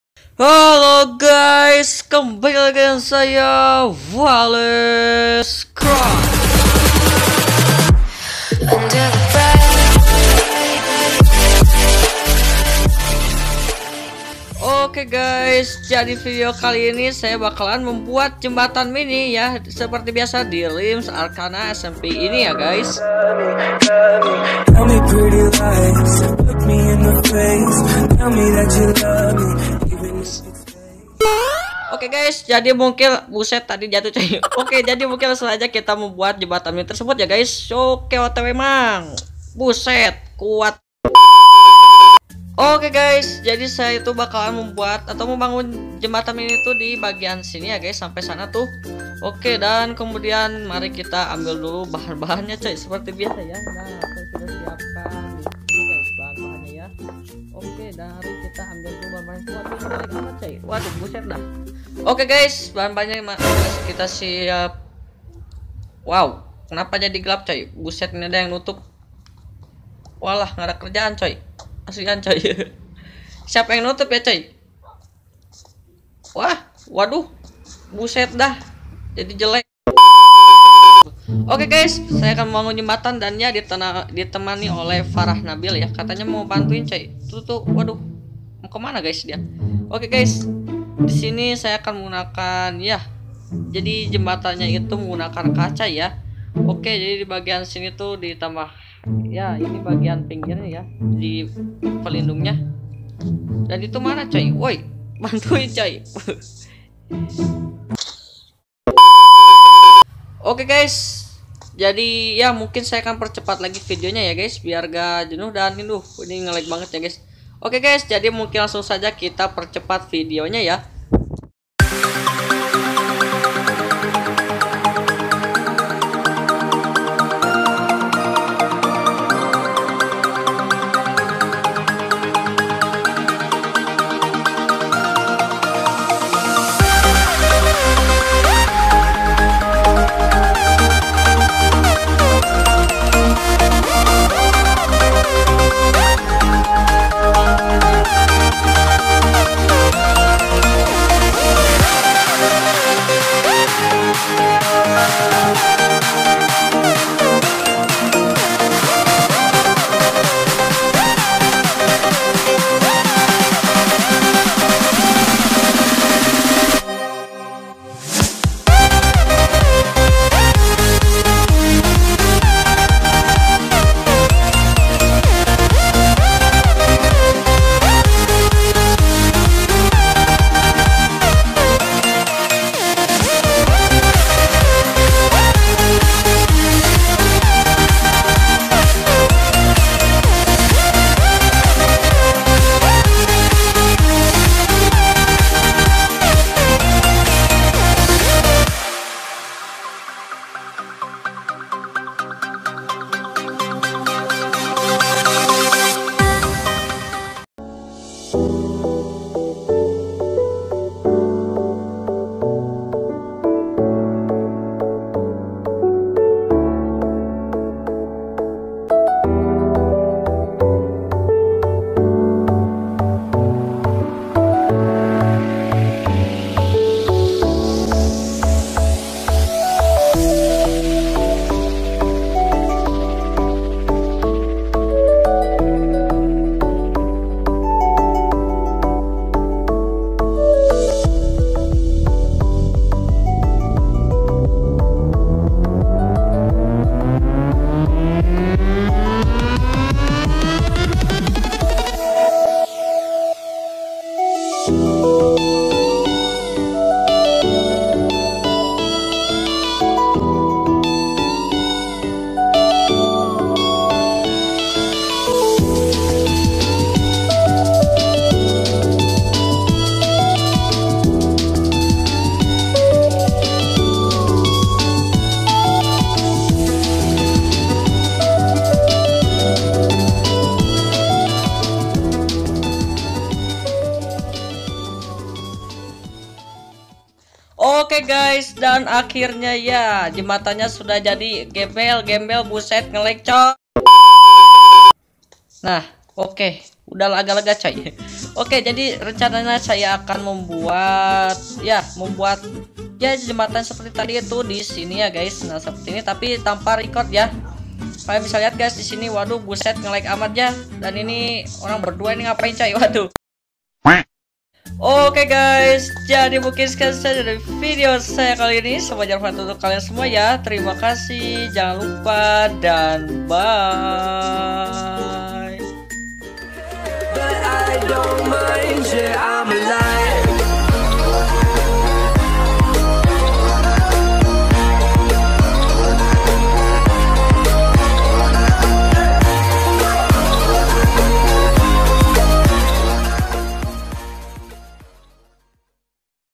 MEMBANGUN JEMBATAN DI ARCANA SMP EPISODE : 3
Hola epribadi

YAP AKHIRNYA SAYA BISA MEMBANGUN JEMBATAN ANTI GALAU DI REALMS ARCANA SMP INI :v .

PADA MAU IKUTAN YAK :D ?
BAGI KALIAN YANG MUNGKIN INGIN MENGIKUTI REALMS ARCANA SMP TERSEBUT KALIAN BISA CEK DI BAGIAN BAWAH INI.

~ID LINE : glgfernandi

BYE!